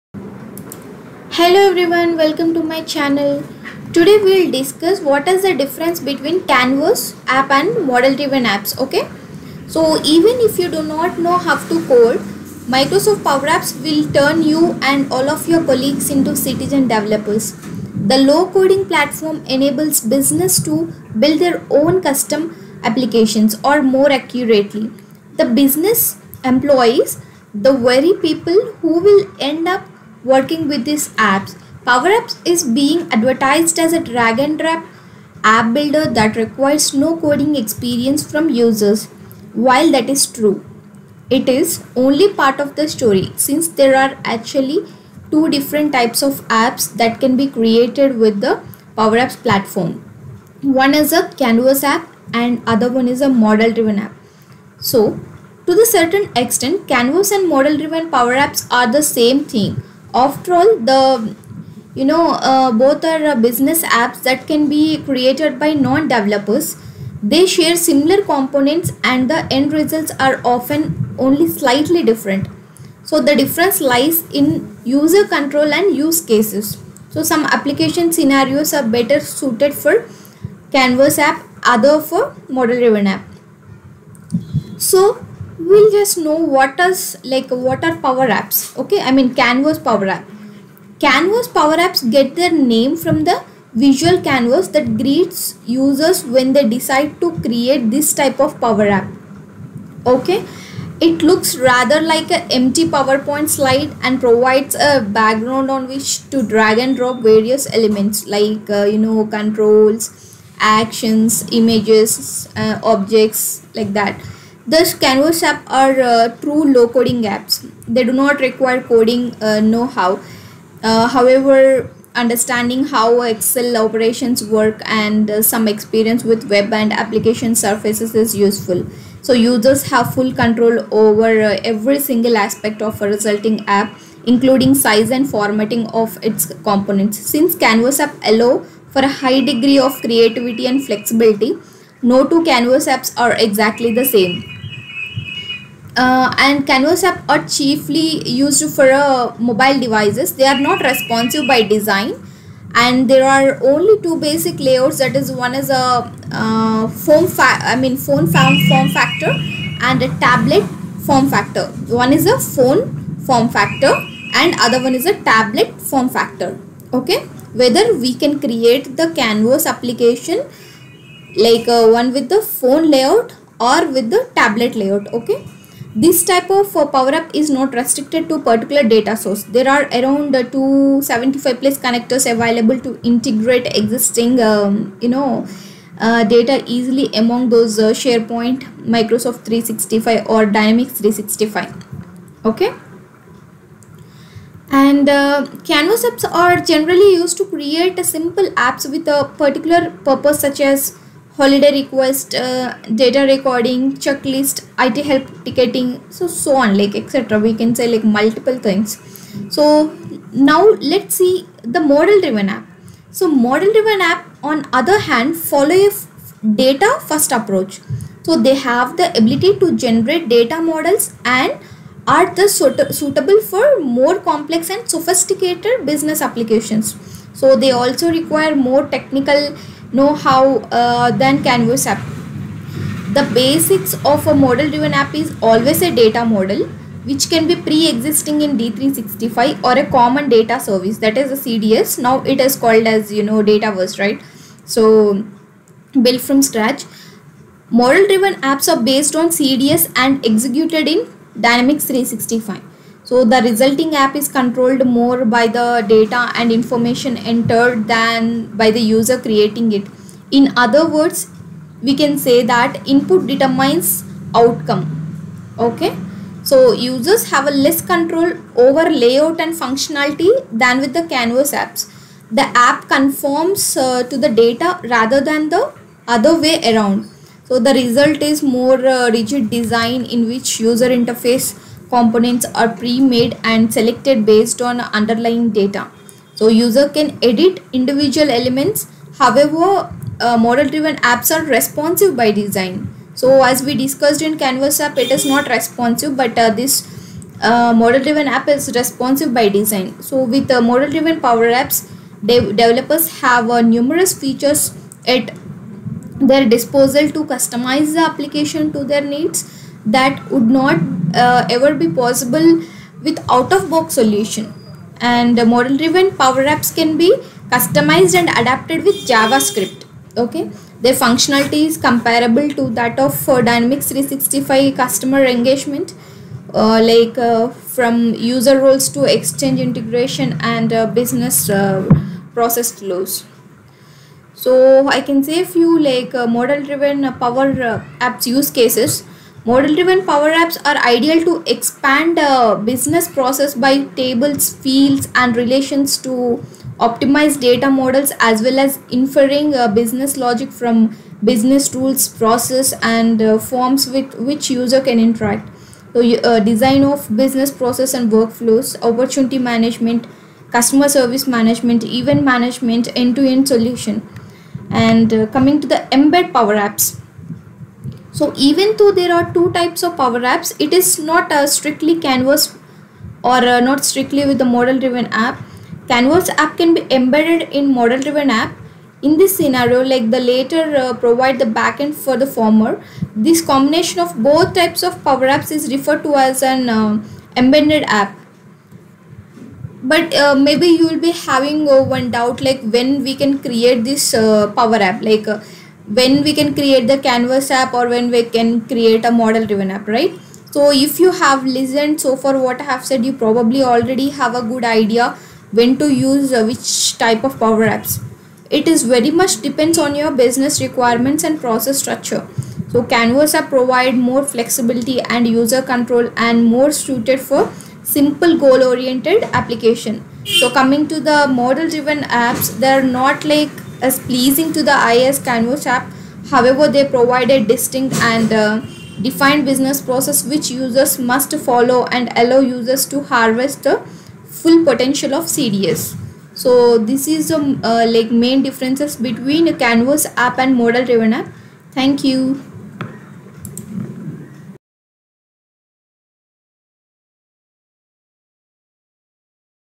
Difference between Canvas and Model Driven in PowerApps | Nowledge | D365 | Microsoft
In this video you will able to learn Difference between Canvas and Model Driven in PowerApps.